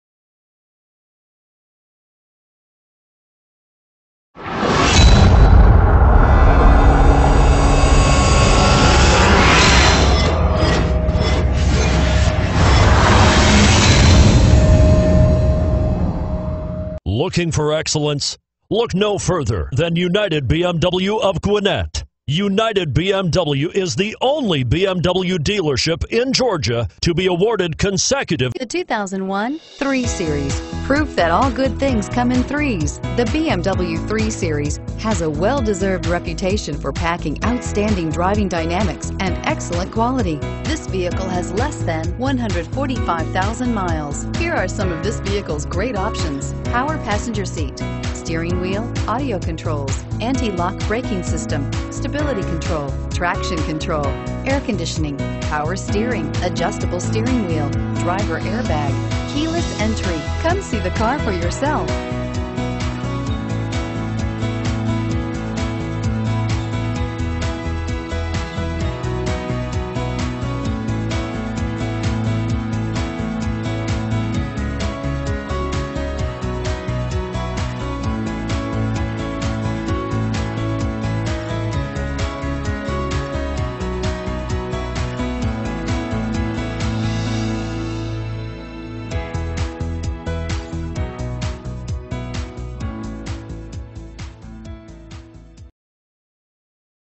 2001 BMW 3 Series Duluth GA F33324A
2001 BMW 3 Series 330i

This 2001 BMW 330i Sedan has a Titanium Silver Metallic exterior with Gray interior!